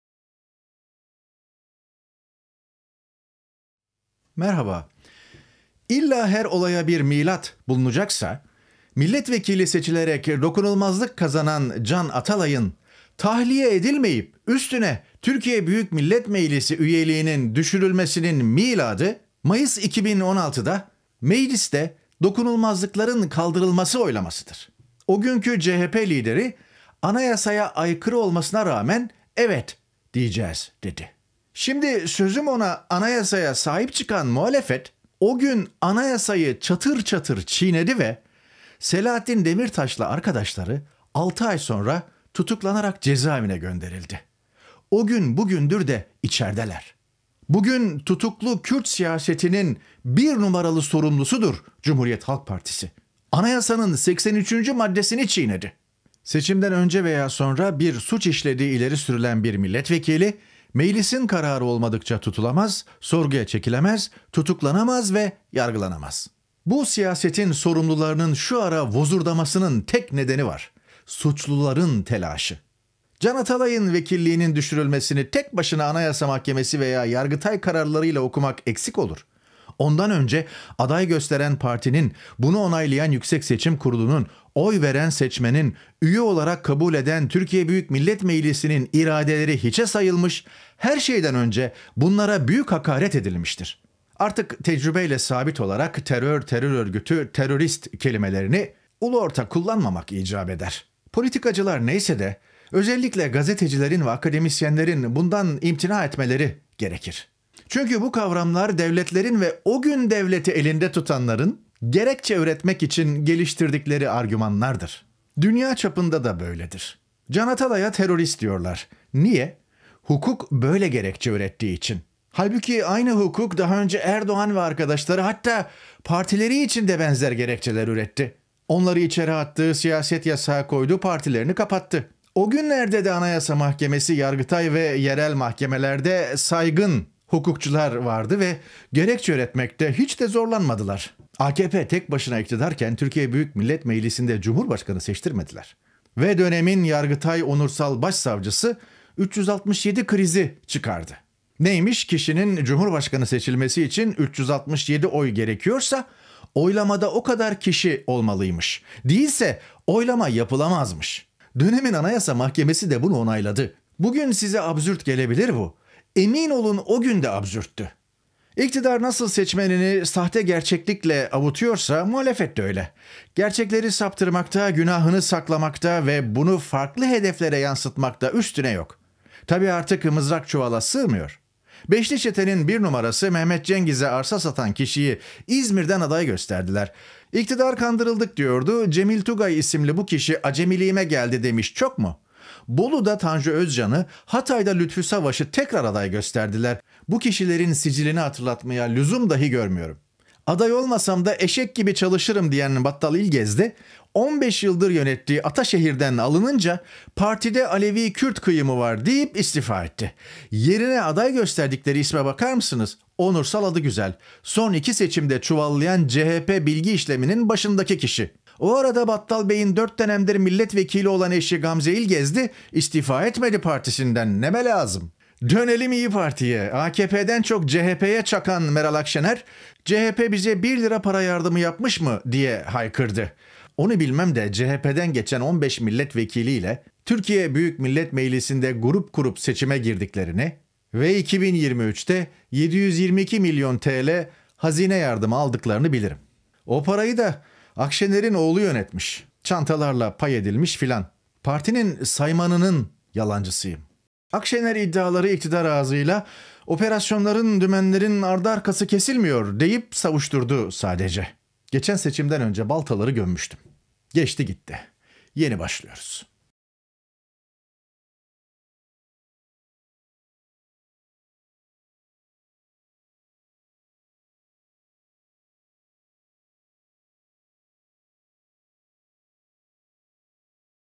Yeni başlıyoruz: Maskelerin düşme vakti.. | Tarık Toros | Analiz | 5 Şubat 2024
5 Şubat 2024

• İlla her olaya bir milat bulunacaksa milletvekili seçilerek dokunulmazlık kazanan Can Atalay’ın tahliye edilmeyip üstüne TBMM üyeliğinin düşürülmesinin miladı, Mayıs 2016’da TBMM’deki dokunulmazlıkların kaldırılması oylamasıdır.
• O günkü CHP lideri, “Anayasa’ya aykırı olmasına rağmen ‘evet’ diyeceğiz” dedi.
• Şimdi sözümona Anayasa’ya sahip çıkan ana muhalefet, o gün çatır çatır çiğnedi ve Selahattin Demirtaş ile arkadaşları 6 ay sonra tutuklanarak cezaevine gönderildi.
• Bugün tutuklu Kürt siyasetinin 1 numaralı sorumlusudur CHP, Anayasa’nın 83’üncü maddesini çiğnedi
• Bu siyasetin sorumlularının şu ara vozurdamasının tek nedeni var: Suçluların telaşı.
• İktidar nasıl seçmenini sahte gerçeklikle avutuyorsa muhalefet de öyle. Gerçekleri saptırmakta, günahını saklamakta ve bunu farklı hedeflere yansıtmakta üstüne yok.
• Geçen seçimden önce baltaları gömmüştüm.
• Geçti gitti. Yeni başlıyoruz.